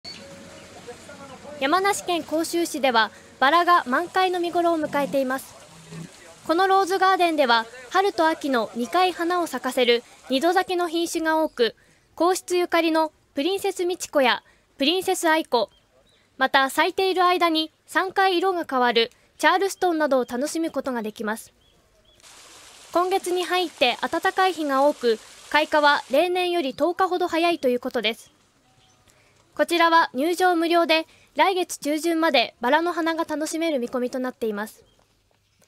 バラ満開の見頃 皇室ゆかり「プリンセスミチコ」も(18/05/12)
山梨県甲州市ではバラが満開の見頃を迎えています。このローズガーデンでは春と秋の2回花を咲かせる「二度咲き」の品種が多く、皇室ゆかりの「プリンセスミチコ」や「プリンセスアイコ」、また、咲いている間に3回、色が変わる「チャールストン」などを楽しむことができます。